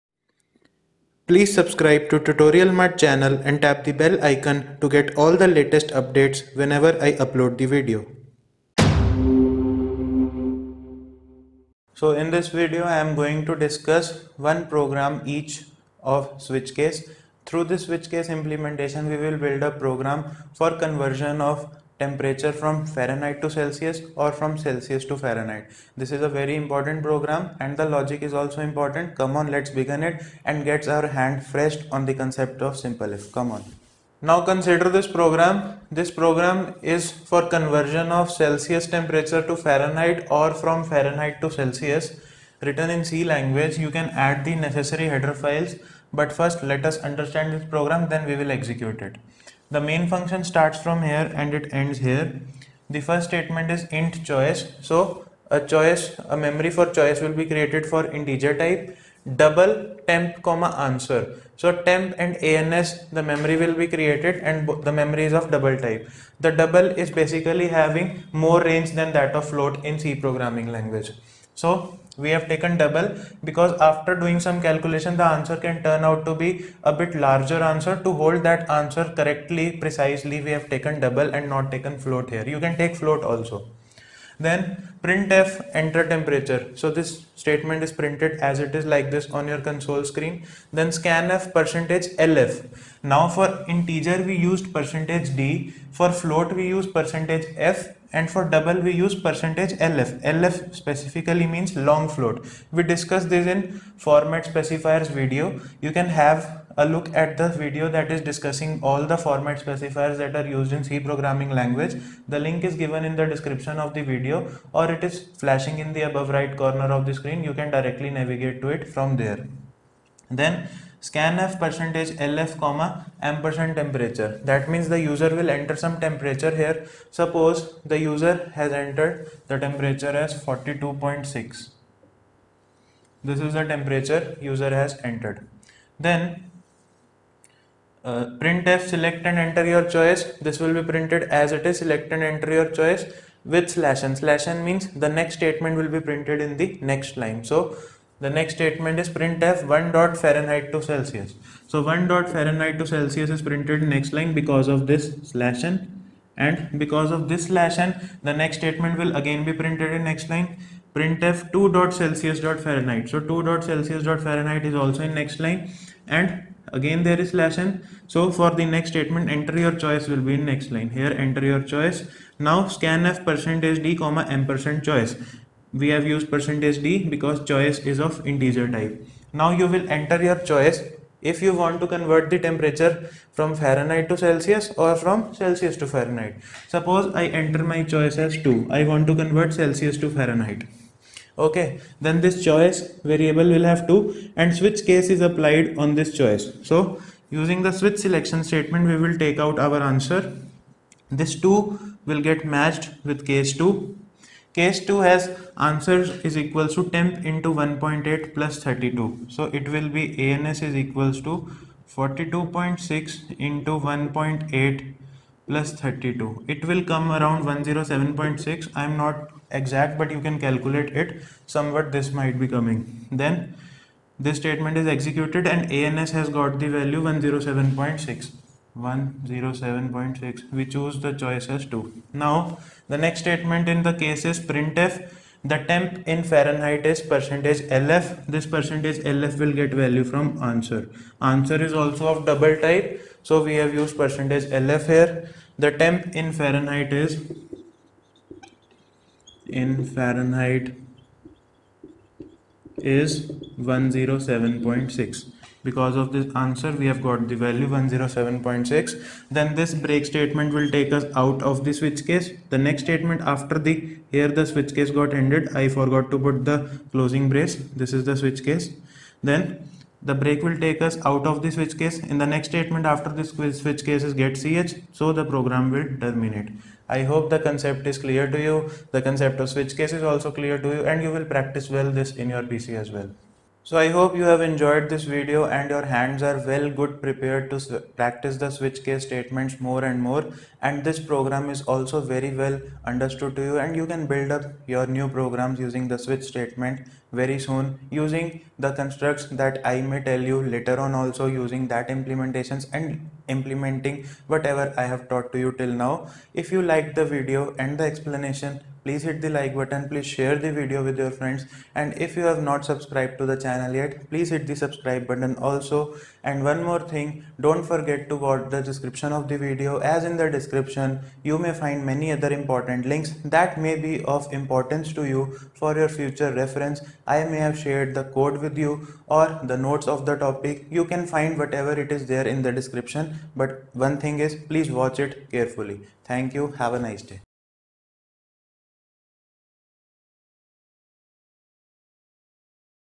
C Program for conversion of temperature from celsius to farenheit and vice versa using switch case
In this video, I have discussed about the program for conversion of temperature from celsius to farenheit and from farenheit to celsius using switch case statement in c programming language.

Time laps : -

1. Program to convert celsius to farenheit  & vice versa - 00:34